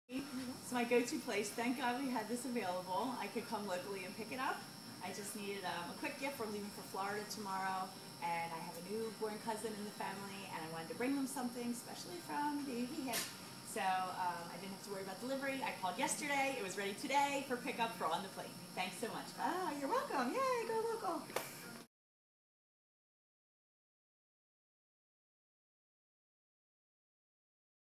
Baby Be Hip's I Love Philly Program!
Go Local with Baby Be Hip! As part of our new "I Love Philly" program, we're offering next day pick up to our nearby customers!